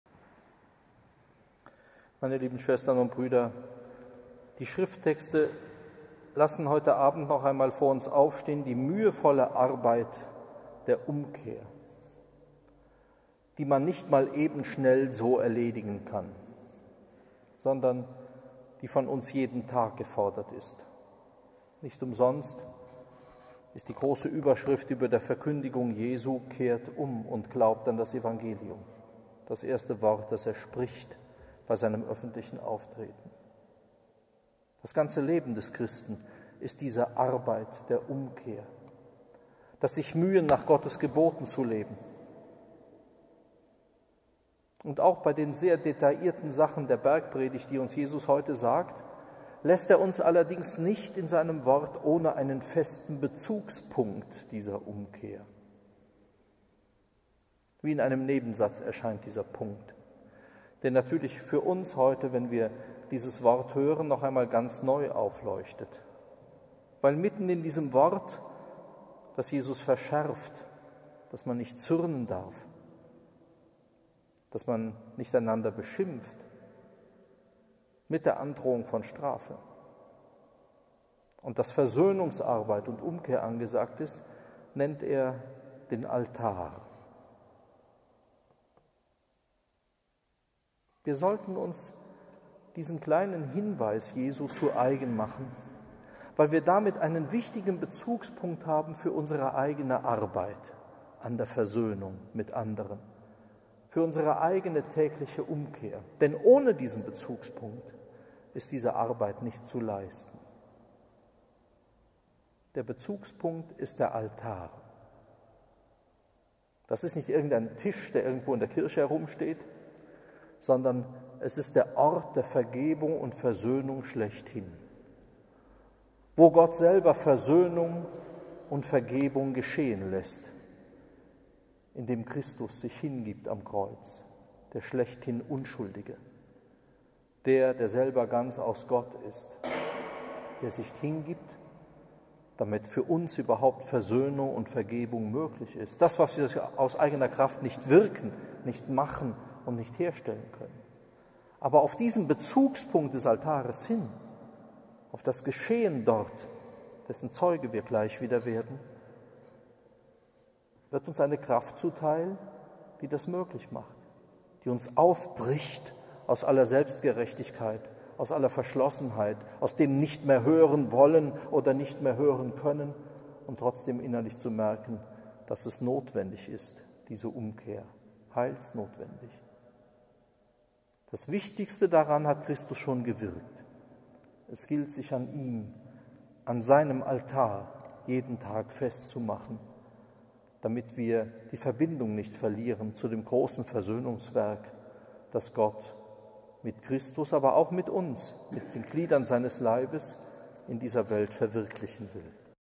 Die mühevolle Arbeit der Umkehr
Predigt von Hochschulpfarrer Dr. Johannes J. Kreier am Freitag der 1.Fastenwoche im Kloster am Rande der Stadt/ Saarbrücken.